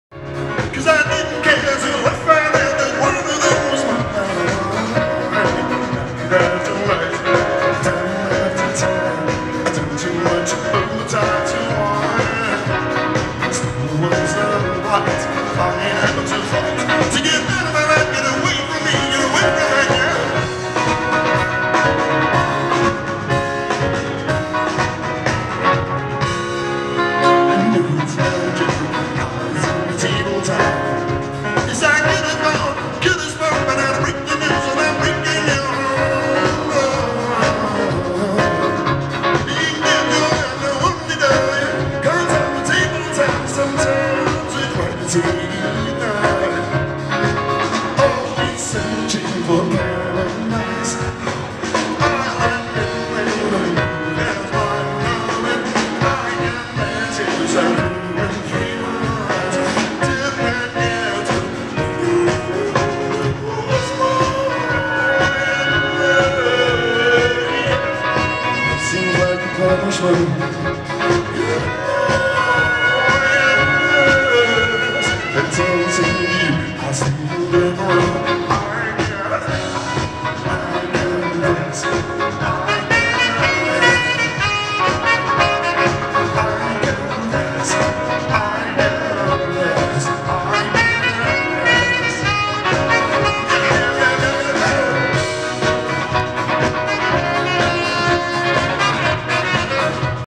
1.28.2017 - ENGLISH BEAT in concert 80's WEEKEND # 3 @ MICROSOFT THEATER IN LOS ANGELES CALIFORNIA
RICKY VANFLEET ENJOYING THE 80's WEEKEND # 3.